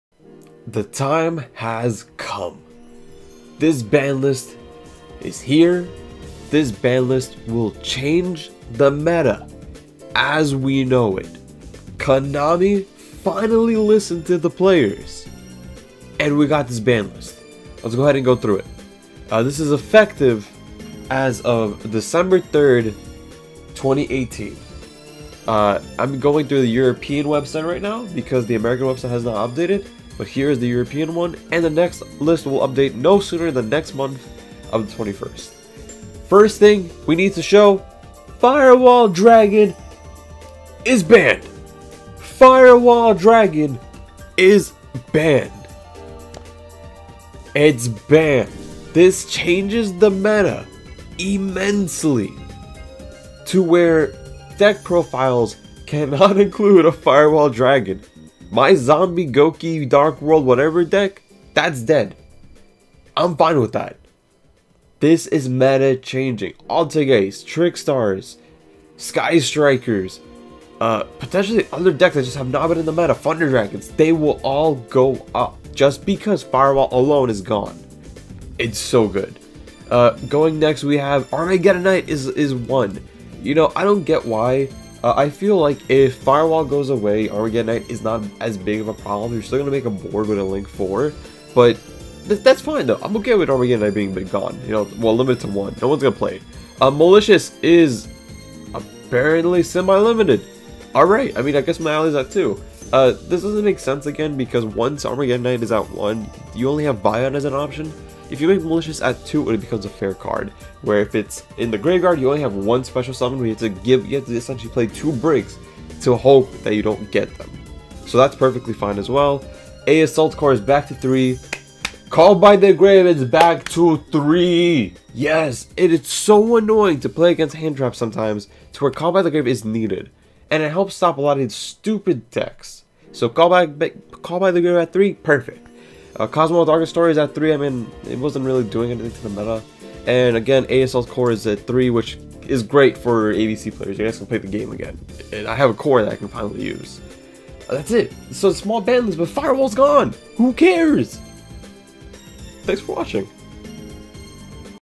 Yu-Gi-Oh! December Banlist - FIREWALL IS BANNED
HUZZAH.
REJOICE.

We can play the game again but Sky Striker and Altergeist are busted.